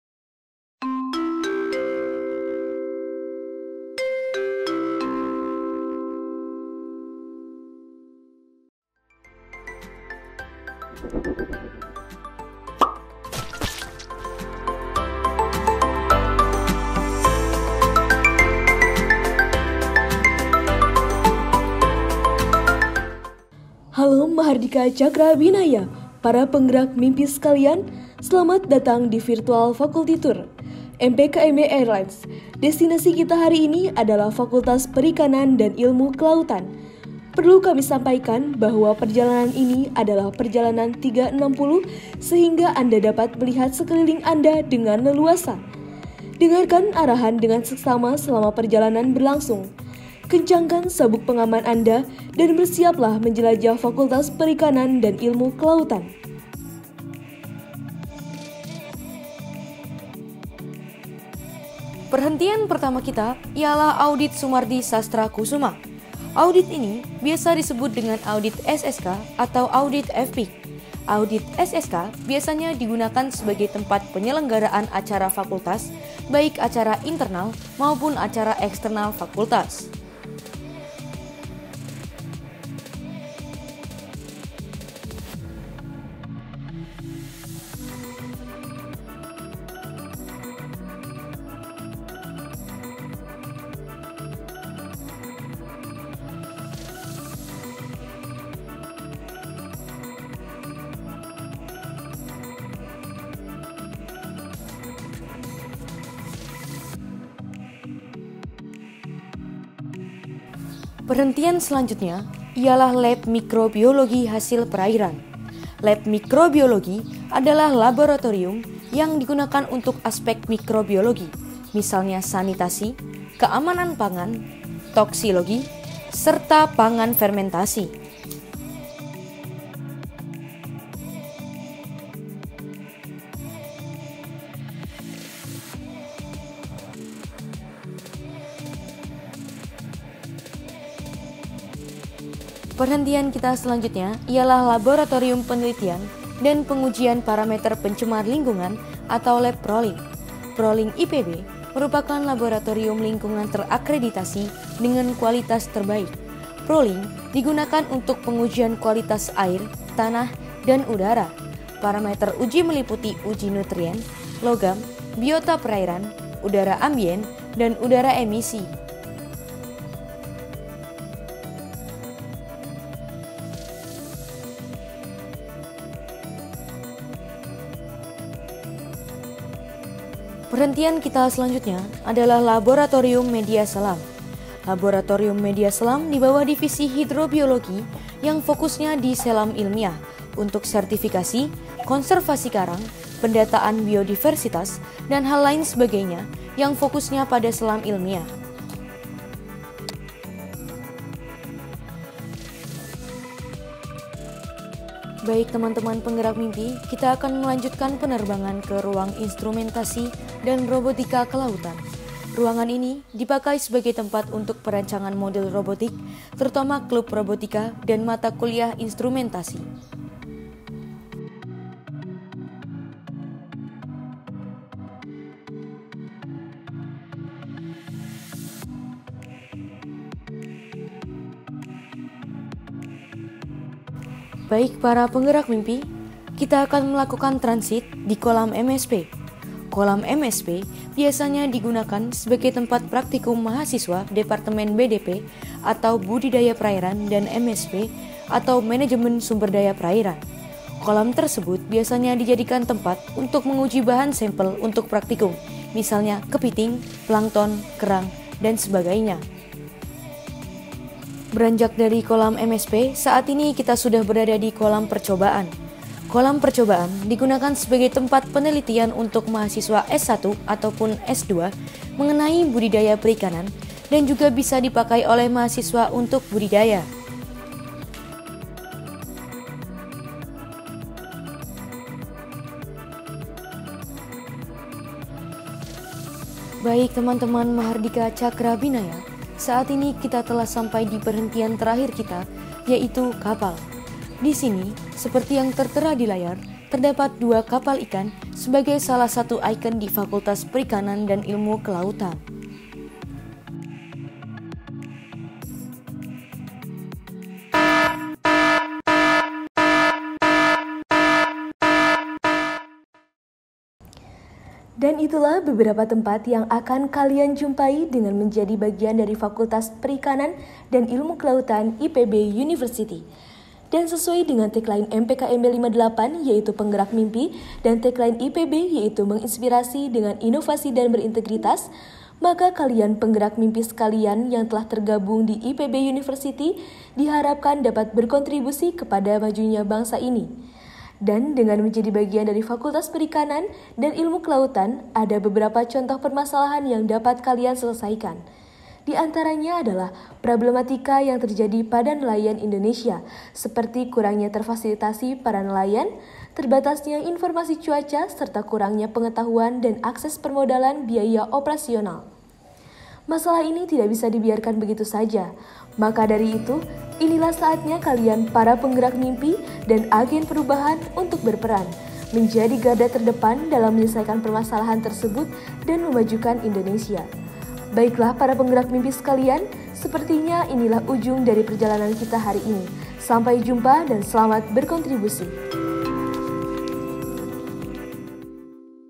MPKMB Airlines - Fakultas Perikanan dan Ilmu Kelautan
Halo, Mahardika Cakrabinaya!
MPKMB Airlines dengan konsep Virtual Tour 360 hadir untuk membawamu berkeliling di tempat-tempat unggulan dan fakultas-fakultas yang ada di IPB University. Petualangan bersama melalui konten ini diharapkan dapat mengenalkan tempat unggulan dan fakultas dari masing-masing mahasiswa baru.

Di video kali ini kita akan mengenalkan Fakultas Perikanan dan Ilmu Kelautan, mulai dari Kolam MSP, dan spot menarik lainnya yang ada di Fakultas Perikanan dan Ilmu Kelautan. Yuk simak videonya!

“Satukan asa dalam wujudkan mimpi, bergerak bersama MPKMB”

Narahubung:

Terima Kasih
Masa Pengenalan Kampus Mahasiswa Baru